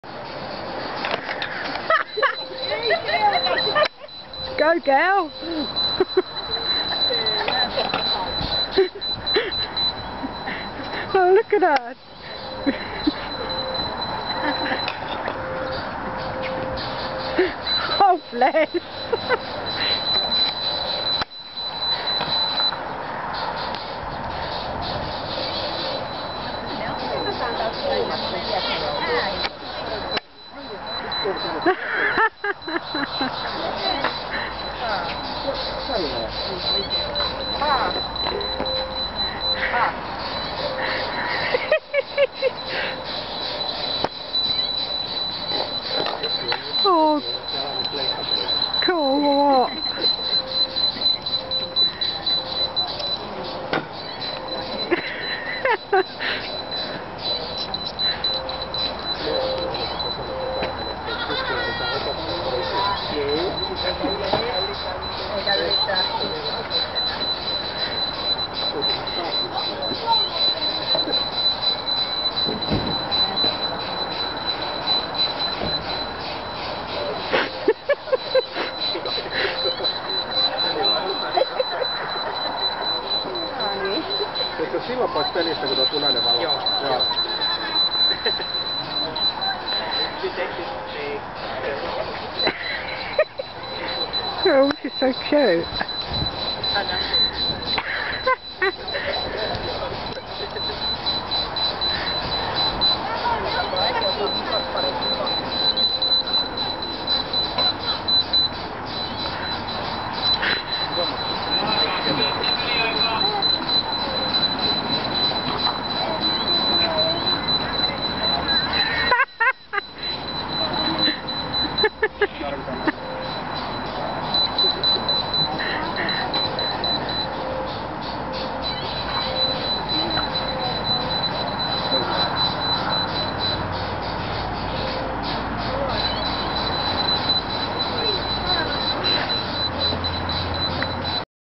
Jasmin having a go at the laser gun
A fun day out  May  3015